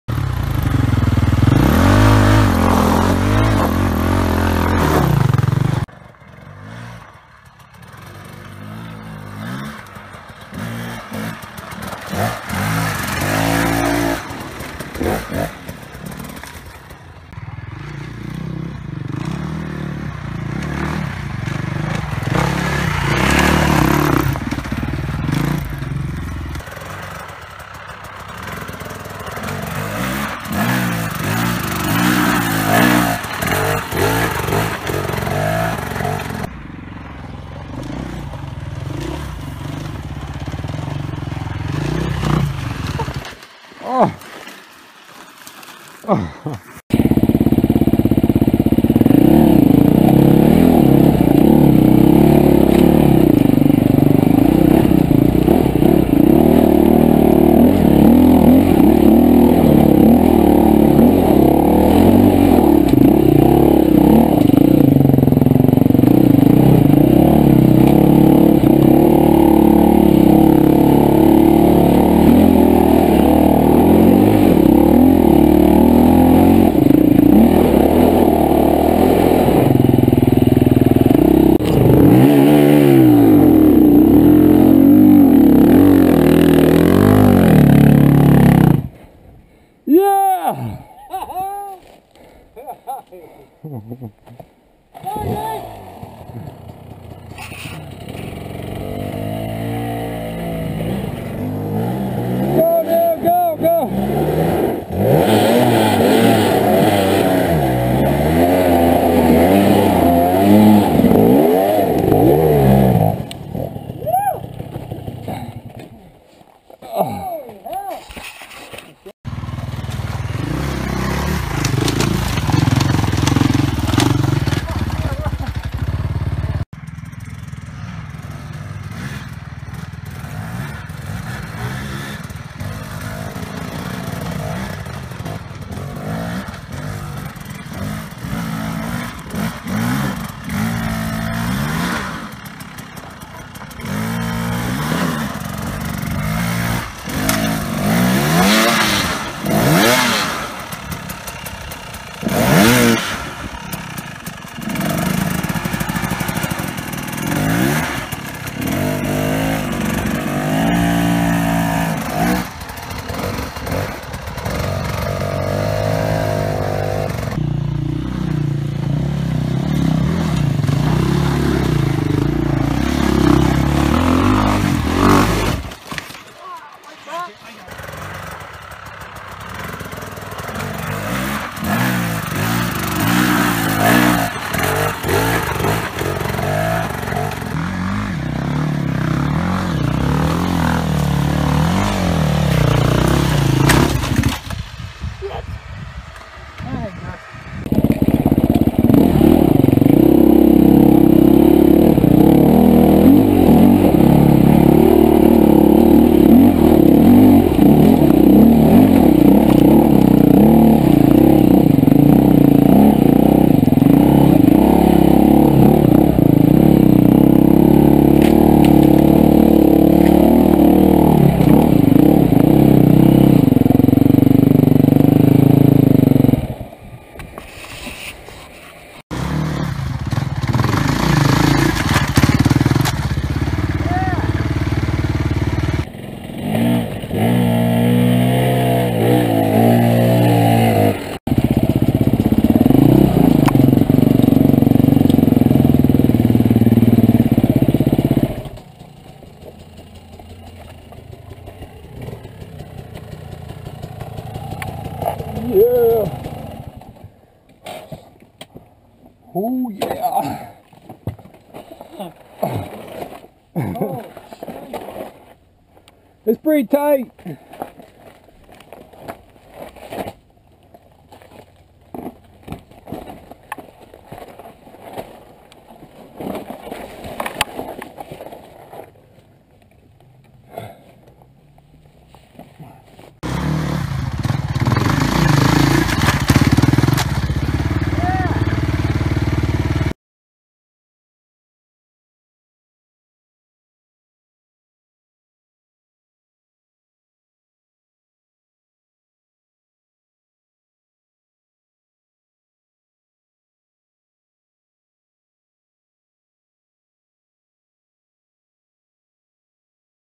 BDM with Dave & Dan
A tough moto ride with lots of boulders and loose decomposed granite!  We call it Boulders Death March.  26 mile loop took all 7 hours.   November 2014